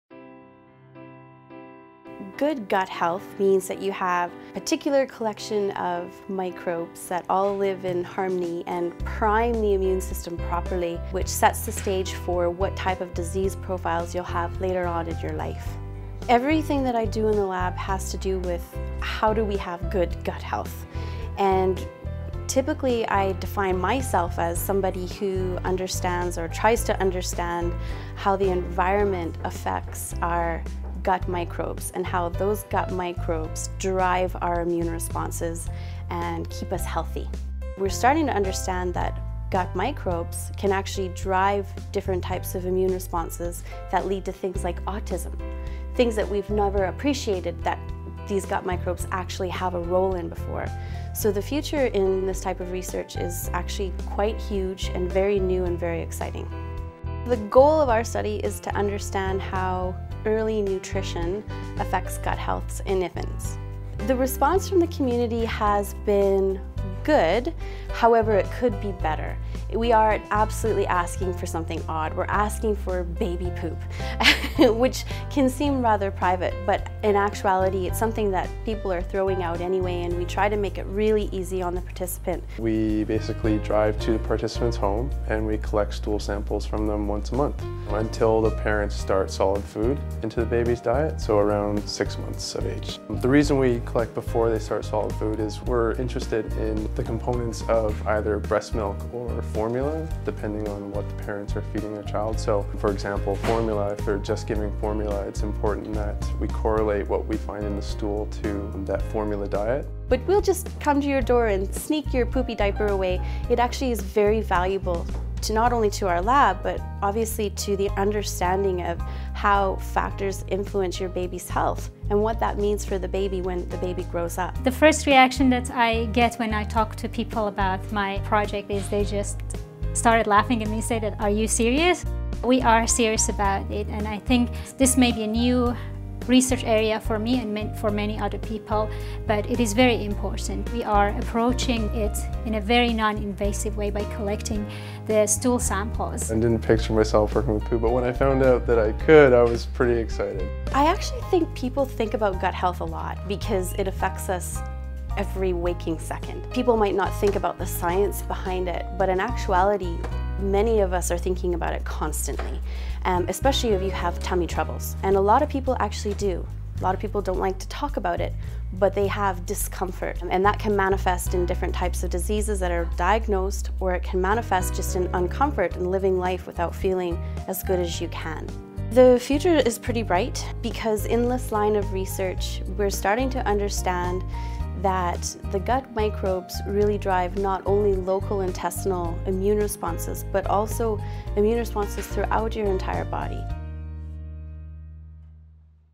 Studying Intestinal Microbiome
The Center of Microbiome and Inflammatory Disease (CMID) laboratory, led by Professor Deanna Gibson, investigates the emerging and exciting field of microbiome research that evaluates the factors which influence microbiota establishment in mammals.